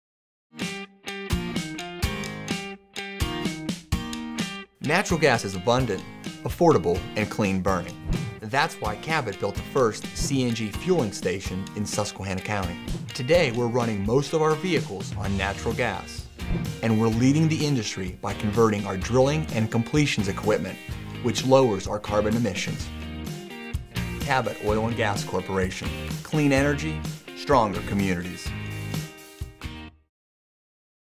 Cabot - "Natural Gas Usage" TV :30
Produced for Cabot Oil & Gas Corporation by Condron & Cosgrove. to learn more..